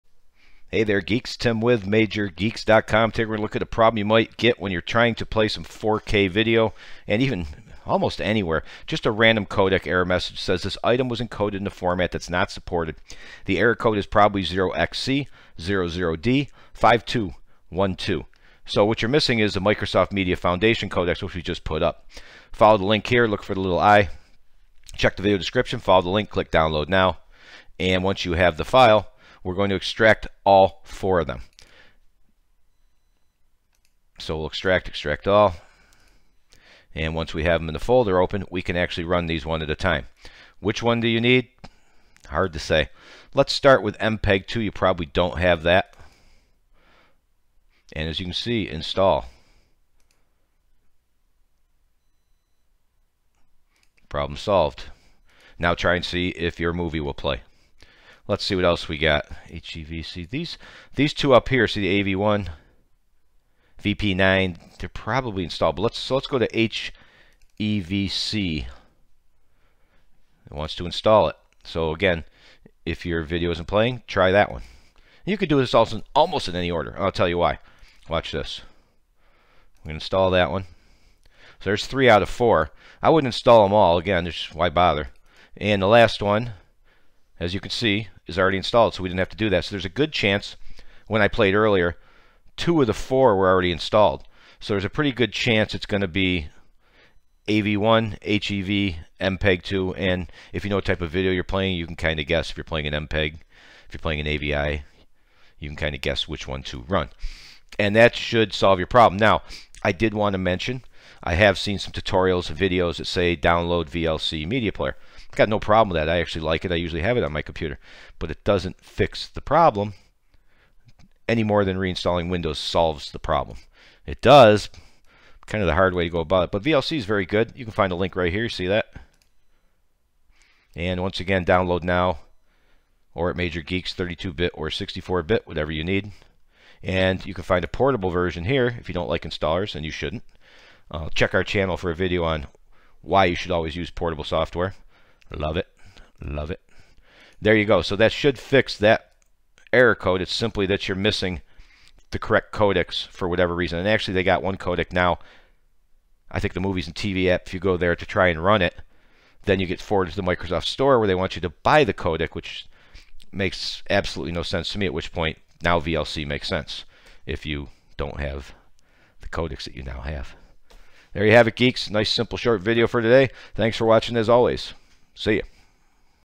0xc00d5212 - This item was encoded in a format that's not supported
Microsoft Media Foundation Codecs contains four possible missing codecs that cause the "This item was encoded in a format that's not supported" that appears primarily in the Movies & TV App when playing 4K videos